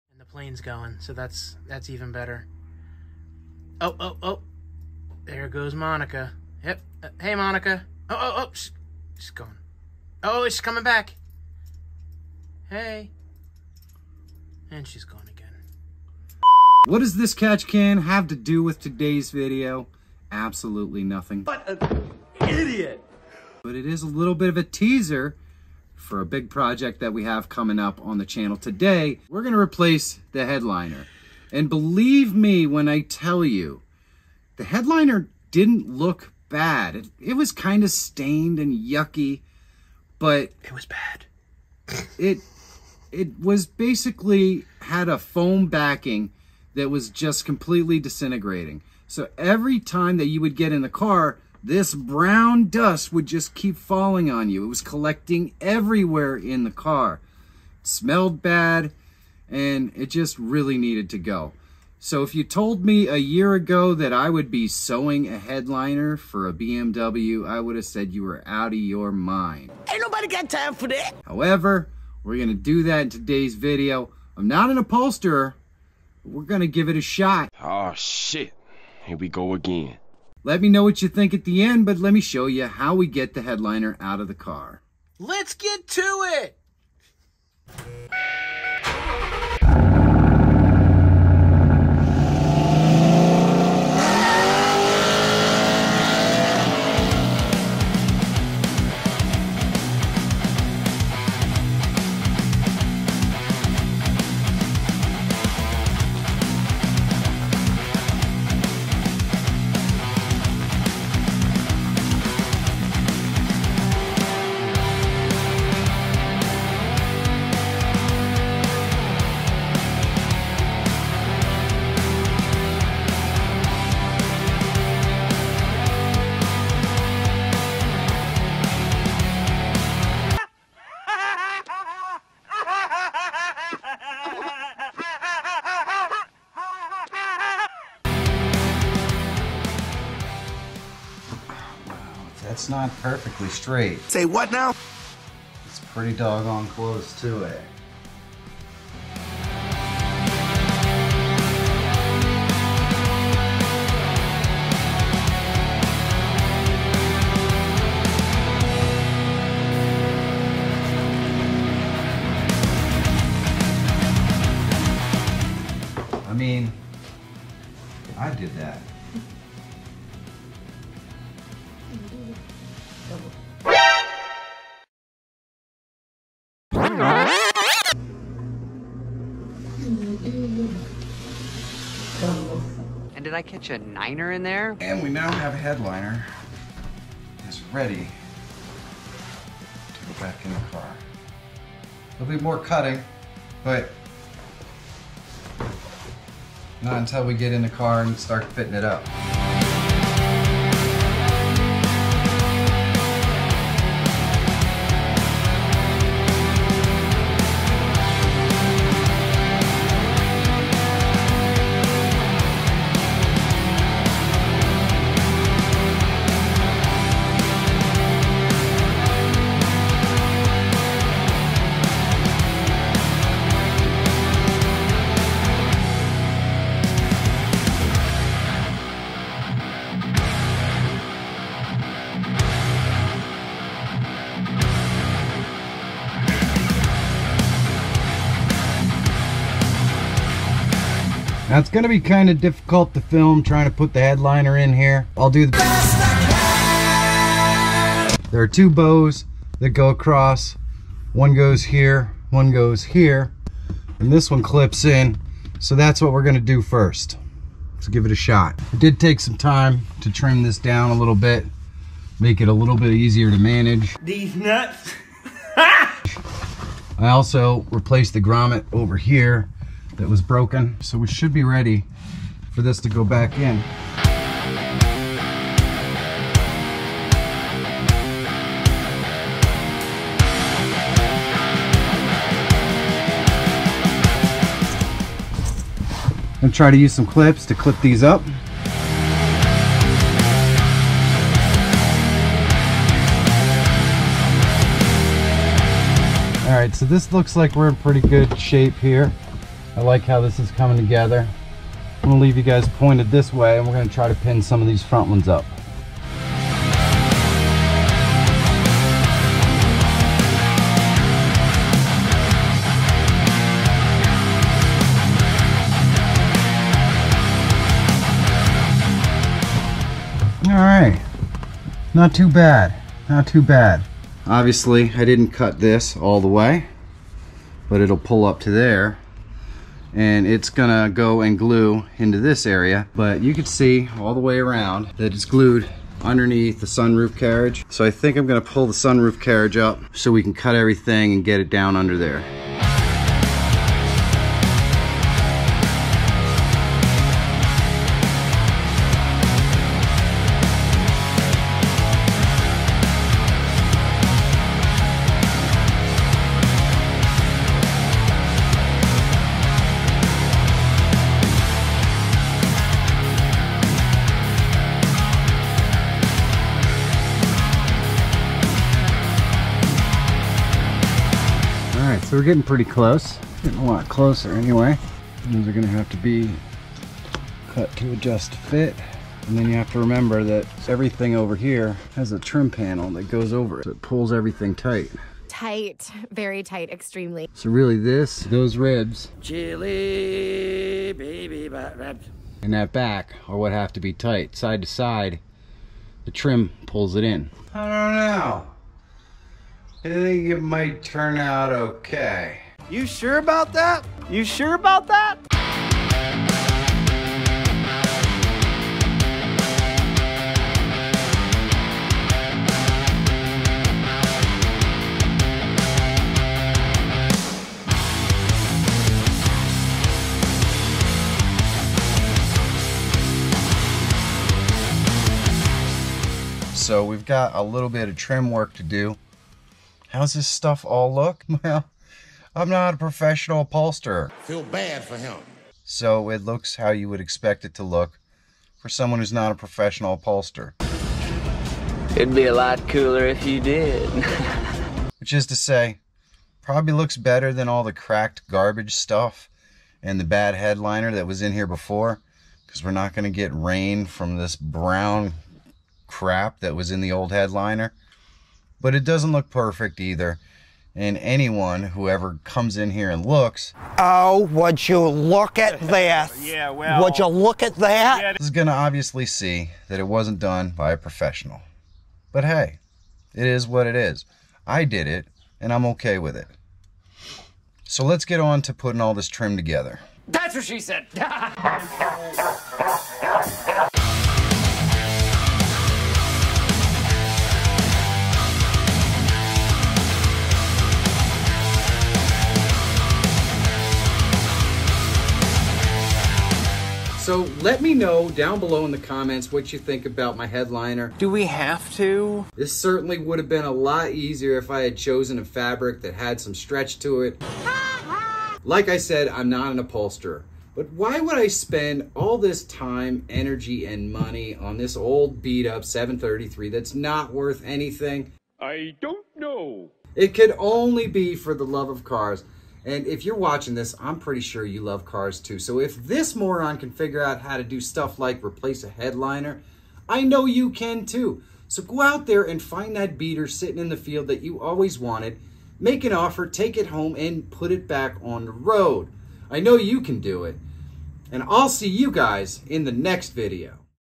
Idiot Makes New Car Headliner and installs on a BMW E23 733i
That's right I sew here. I didn't see it coming either. How does it turn out? You be the judge.